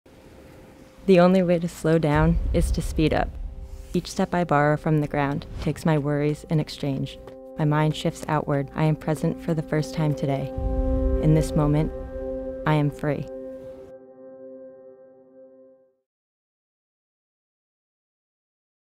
Freedom in Speed: Ciara Roche's Run with Nike Streakfly
Step into a world where speed liberates and every stride is a journey towards freedom. Presenting the Nike Streakfly: a revolution in running, spotlighted through the journey of Ciara Roche, a luminary in the New Jersey Running community. This powerful advertisement, crafted with precision and artistry by Loud Media, is elevated by the resonant words of Amanda Huneke's poem, capturing the essence of running not just as a physical activity, but as a spiritual experience.

👟 Empowerment with Every Step 👟
The Nike Streakfly is not just a shoe; it's a statement. For Ciara Roche, it's the key to unlocking a version of herself that is unburdened and unstoppable. Witness as Ciara takes to the streets and trails of New Jersey, her every step in the Streakfly turning worries into wind, showcasing the shoe's exceptional design that promises comfort, responsiveness, and an aesthetic that speaks volumes.

🎥 A Visual Poem of Liberation 🎥
Narrated by the profound verses of Amanda Huneke's poem, this advertisement goes beyond the conventional, weaving a narrative that touches the soul. "The only way to slow down is to speed up," the voice-over begins, guiding viewers through a journey of self-discovery and liberation with Ciara as the embodiment of this transformation.

Film, video, video production, branding, shoe review, sports, athletics, running, Nike, community. These keywords are the pillars of this campaign, guiding viewers to a story of personal breakthrough and community connection. It's a celebration of the spirit of running, shared by Ciara and echoed by runners everywhere who find freedom in the rhythm of their steps.

🗣 A Journey Beyond the Physical 🗣
This advertisement is a homage to the power of running to transcend. "Each step I borrow from the ground takes my worries in exchange. My mind shifts outward. I am present for the first time today. In this moment I am free," the poem narrates, capturing Ciara's transcendence through running, a moment of pure presence and freedom that the Nike Streakfly facilitates.

🚀 Be Part of the Movement 🚀
This isn't just about a running shoe; it's an invitation to experience running as a path to freedom. Discover the Nike Streakfly with Ciara Roche and find your own freedom in speed.

Embrace the journey, embrace the liberation. With the Nike Streakfly, every run is an opportunity to break free. 🌈👟